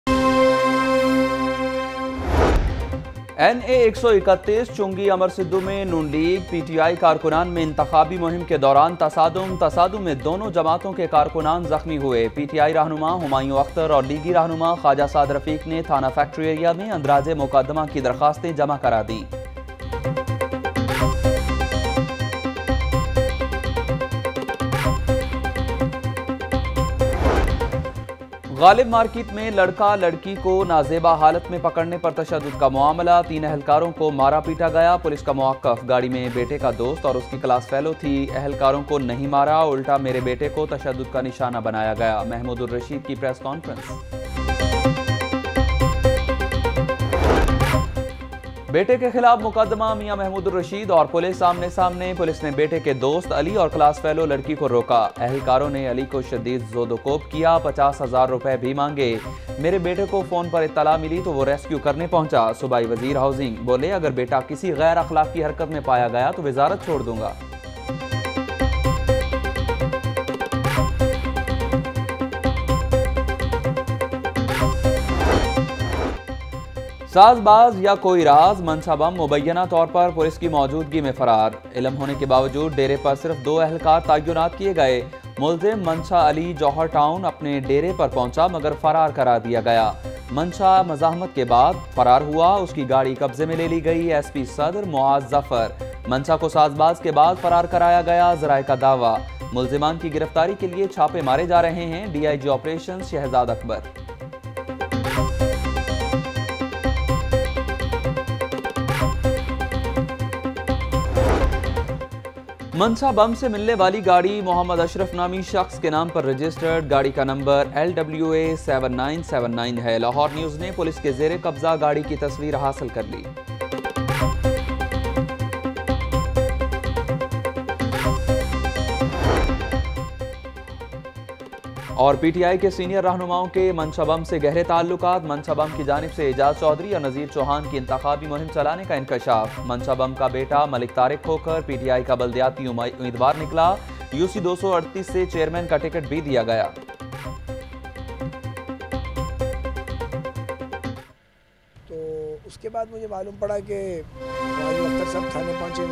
04 AM Headlines Lahore News HD – 05 October 2018
Watch News for PPP diles petition seeking suspension of CM Punjab Usman Buzdar, Punjab Assembly’s first working session to be held today, Rana Sanaullah declared mastermind of Model Town massacre, Arrest warrant of Former IG Punjab Mushtaq Sukhera issued, Rana Mashood responds to controversial statement, Shahbaz Sharif angry on Rana Mashood over his interview, Dr Shahzad Naseem wins by-polls on Senate’s seat, Hamza Shahbaz criticizes PM Imran Khan, Khawaja Saad Rafique blames PTI for price hikes, ICC World Trophy comes Lahore , Police officials torture innocent lady, Scuffle breaks out between locals and traffic wardens, LPG prices witness sharp increase, Oil tanker collides at Waris Meer underpass and much more.